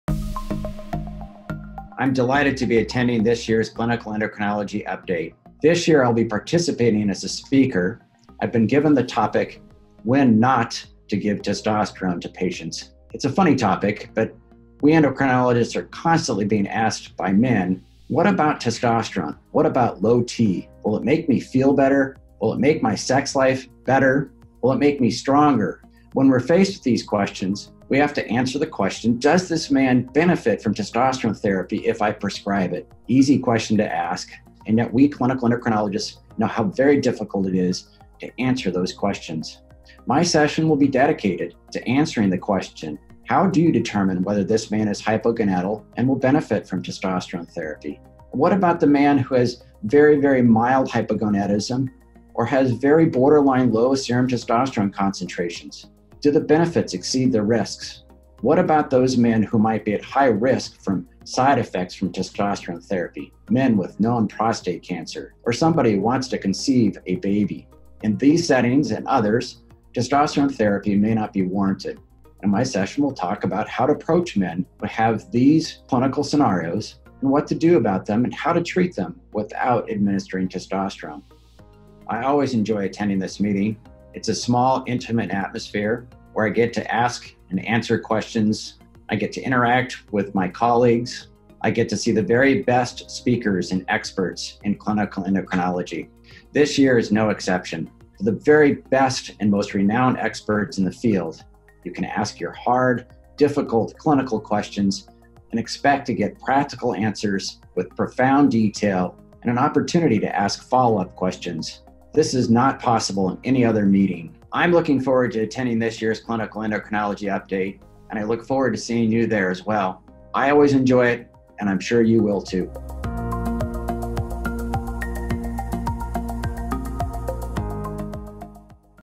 Bradley Anawalt, MD | Testosterone and Hypogonadal Sessions at CEU 2019
Renowned faculty member, Bradley Anawalt, MD, an expert on male reproductive endocrinology, will be presenting at Clinical Endocrinology Update (CEU) in September 2019.

Check out his insights on the meeting as a whole, and learn more about his sessions, When Not to Use Testosterone in Men and Hypogonadal Cases. View the full program